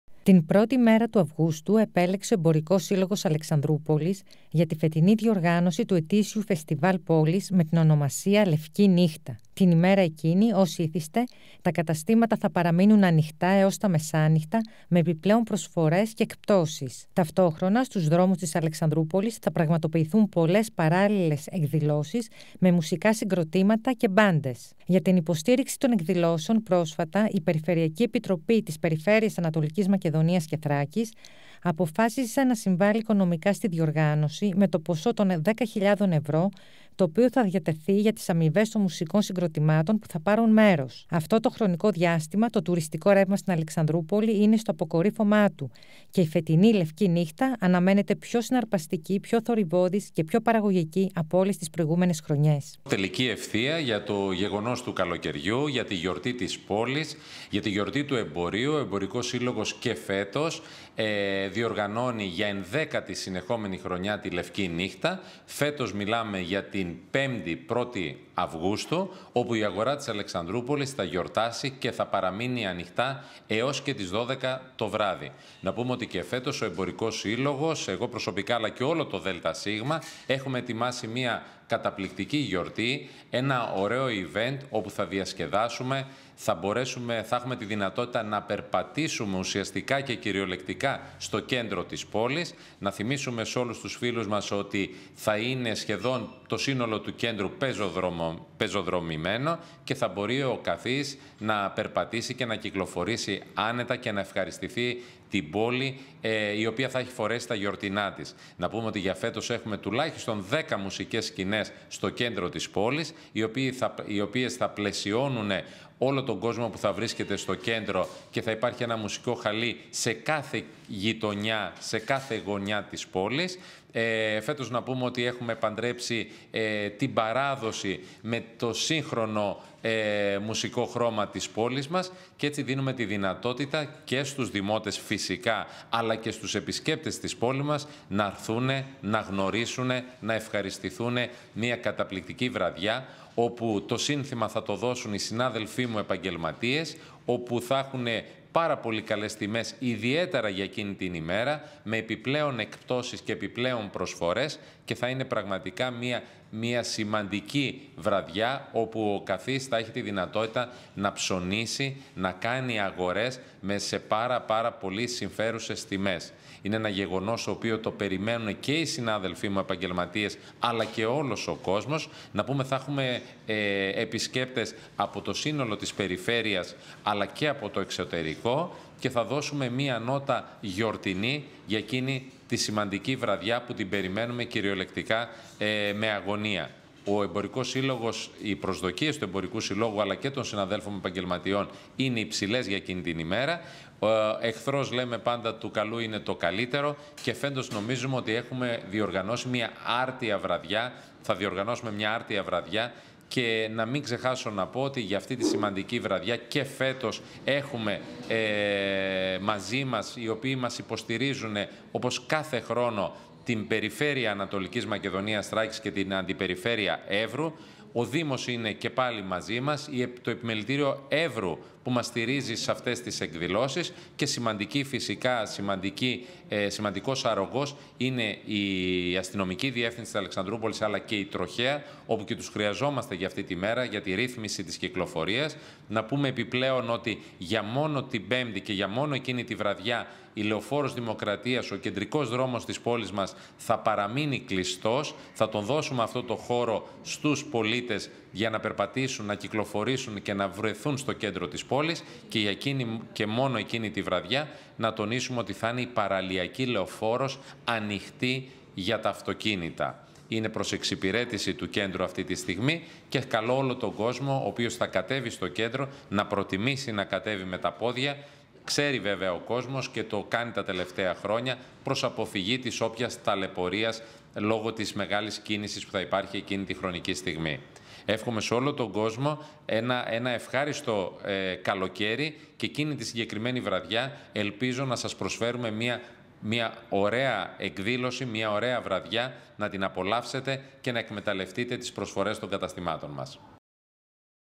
Αλεξ/πολη: Την Πέμπτη 1 Αυγούστου η «Λευκή Νύχτα» - Όλα έτοιμα για το ετήσιο φεστιβάλ
Όλα είναι έτοιμα στην Αλεξανδρούπολη για την μεγάλη γιορτή του εμπορίου, τη "Λευκή Νύχτα", που θα πραγματοποιηθεί την Πέμπτη 1 Αυγούστου.Την ημέρα εκείνη τα καταστήματα θα παραμείνουν ανοιχτά μέχρι της 12 το βράδυ, δίνοντας την ευκαιρία σε όλους τους πολίτες να κάνουν τις αγορές τους σε πολύ προσιτές τιμές. Την εκδήλωση διοργανώνει ο Εμπορικός Σύλλογος της πόλης σε συνεργασία με την Περιφερειακή Ενότητα Έβρου, τον Δήμο, την Τροχαία και το Επιμελητήριο.